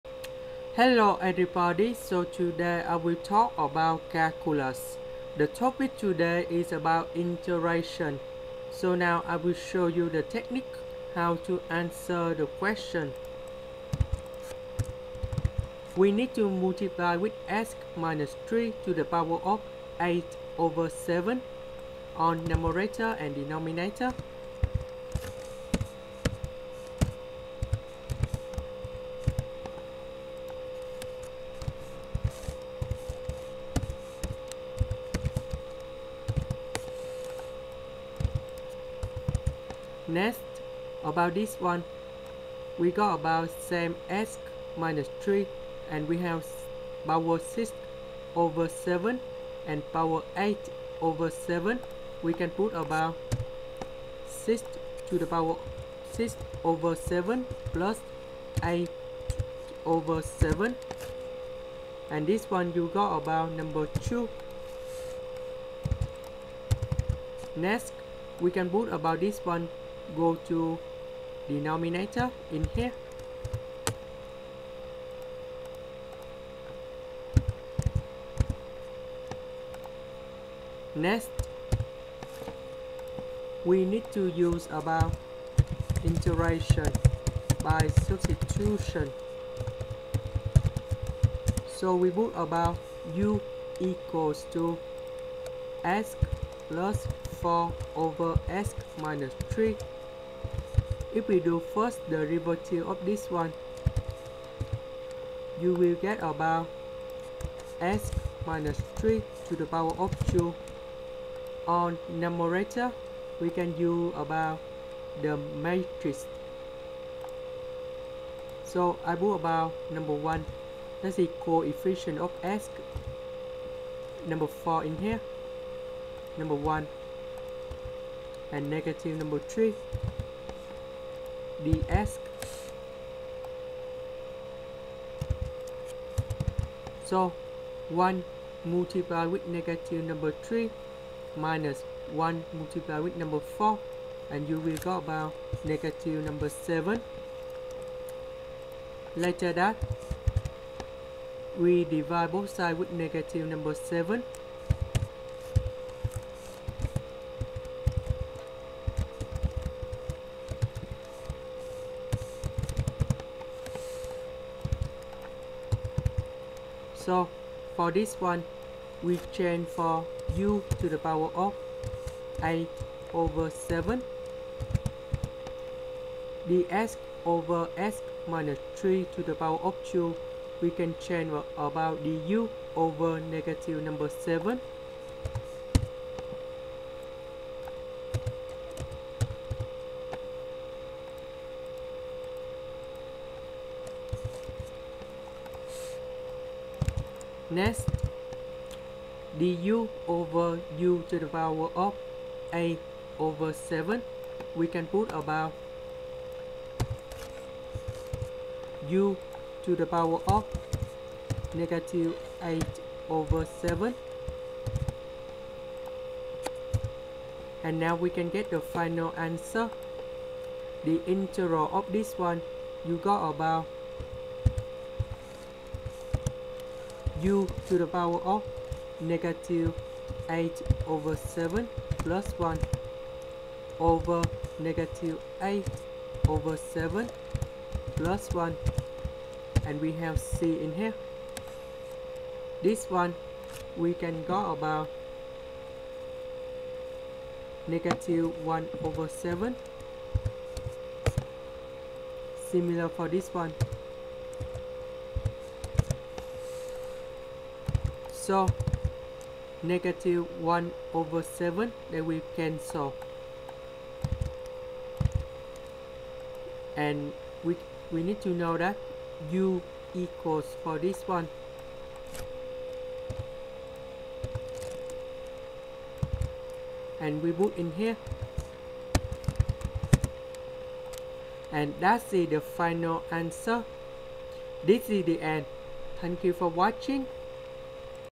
Calculus Help: Integral of dx/((x+4)^(8/7) (x-3)^(6/7) ) - Indian Integration भारतीय एकता
Here is the technique to find this integration and how to solve